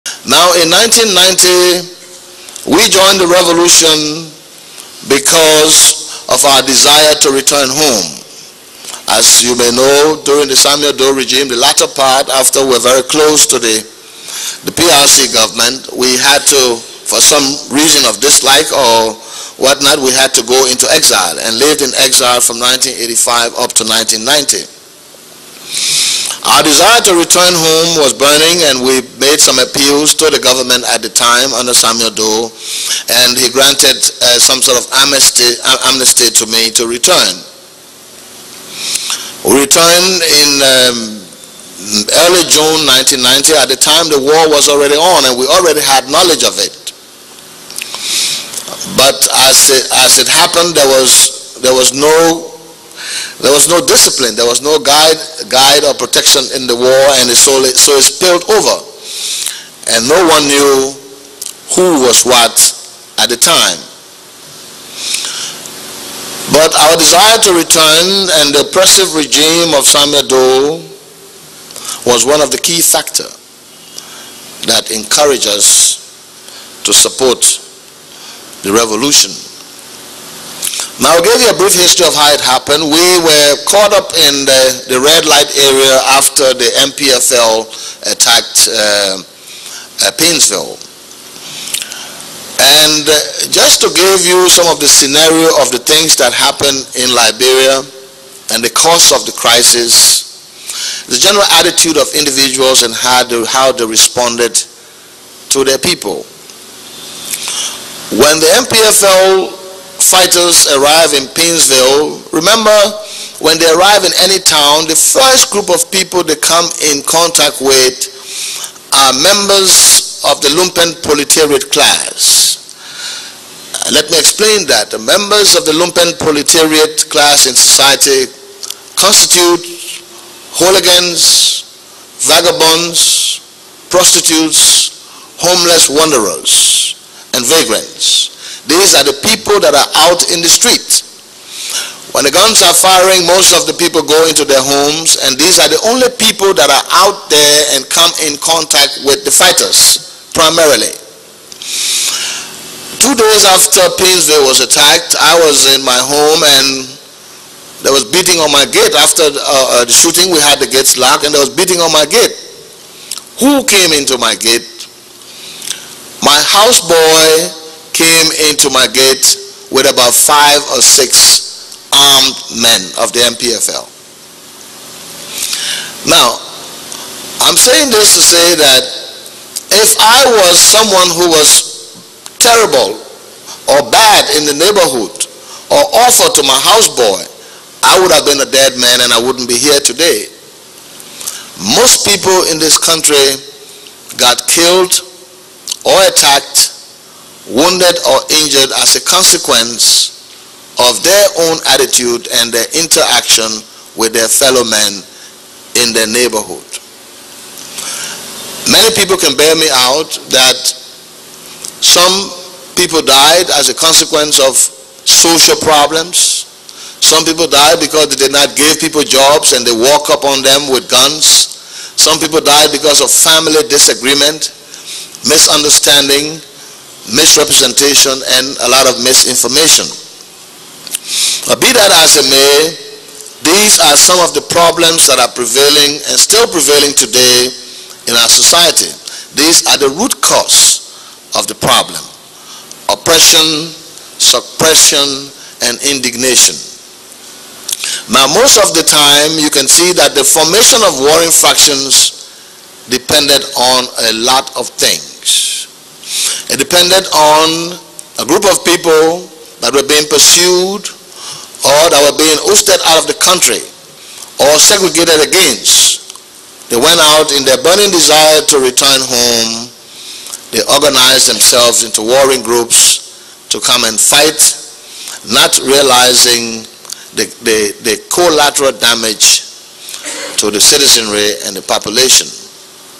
The NPFL Invasion Of Liberia And The Start Of The Civil War (Cyril Allen TRC Testimony) 🇱🇷🇱🇷
The military aspects of NPFL were led by Charles Taylor, a former government official who was being sought for trial on charges of corruption, the NPFL took up arms against the regime of Samuel Doe on 24 December 1989. Most NPFL fighters were originally drawn from the Gio and Mano ethnic groups of northern Liberia who were persecuted under Doe's regime.

Ellen Johnson Sirleaf served as International Coordinator of the NPFL. Taylor and Tom Woewiyu were also in leadership positions.

Martina Johnson was one of the NPFL commanders who is alleged to have “participated directly in mutilation and mass killing in late 1992 during an NPFL offensive known as ‘Operation Octopus’.